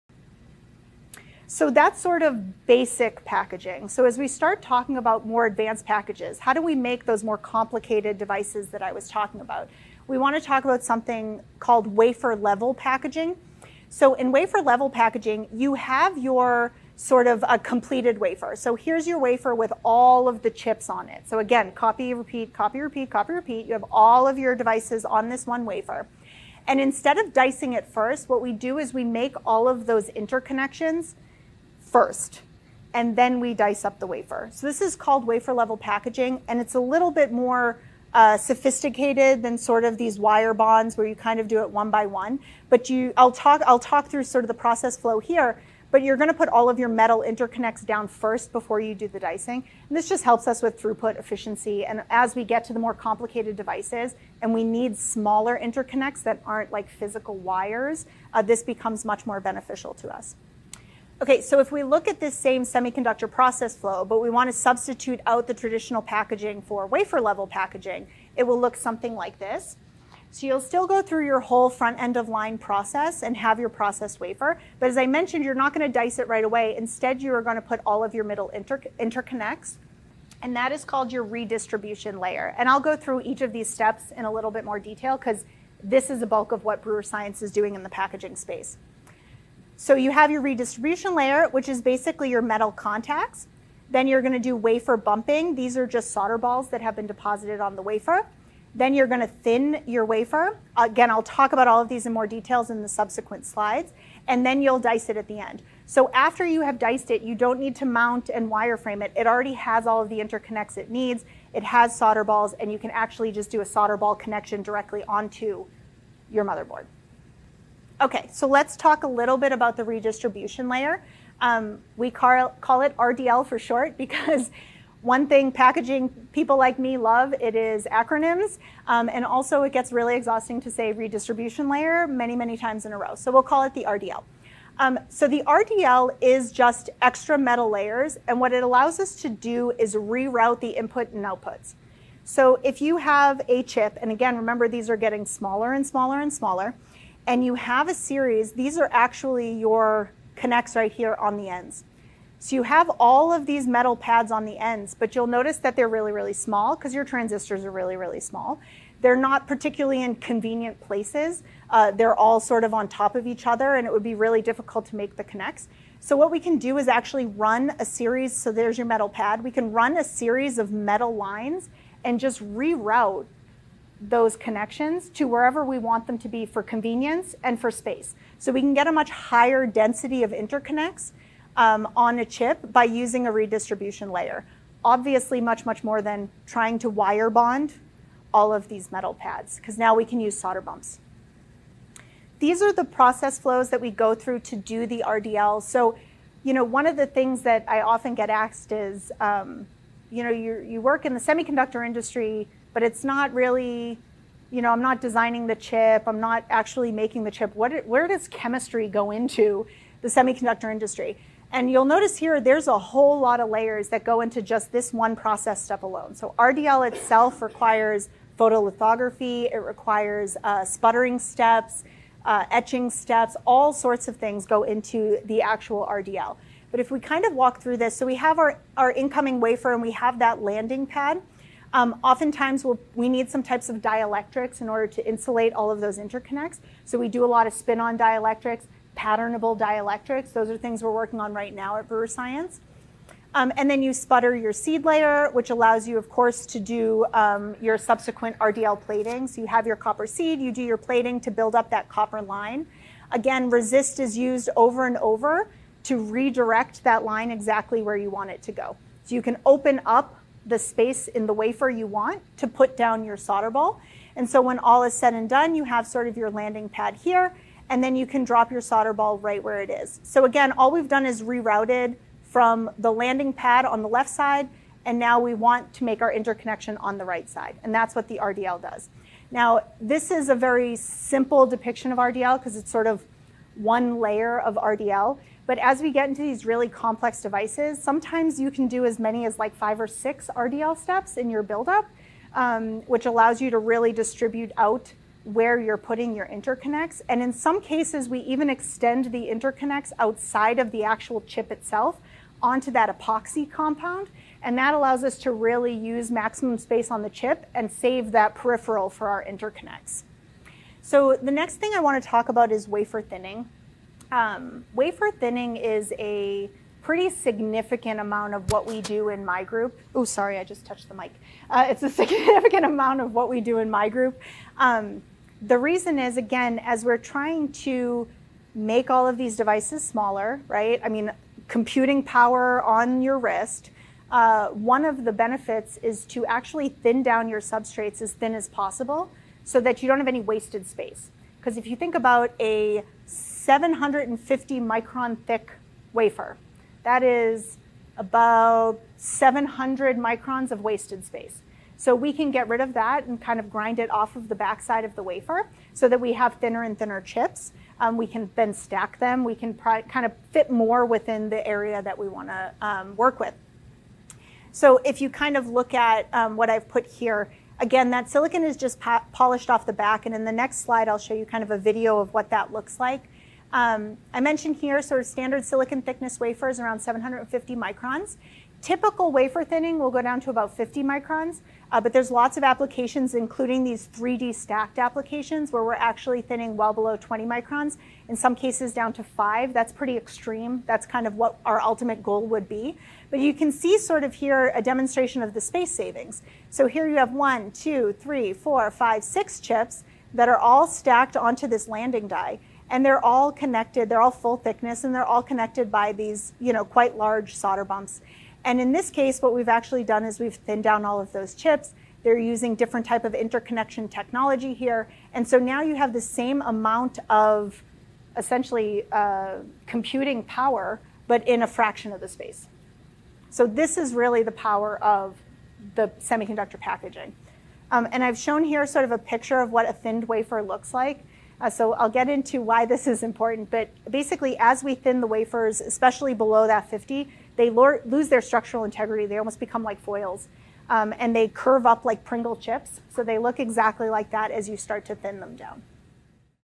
Unveiling Wafer Level Packaging | Moore's Law Series | Part 2
In Part 2  of the Moore's Law series, Andrea explores the intricacies of wafer-level packaging.

Explore Brewer Science's contributions to advanced lithography and innovative packaging solutions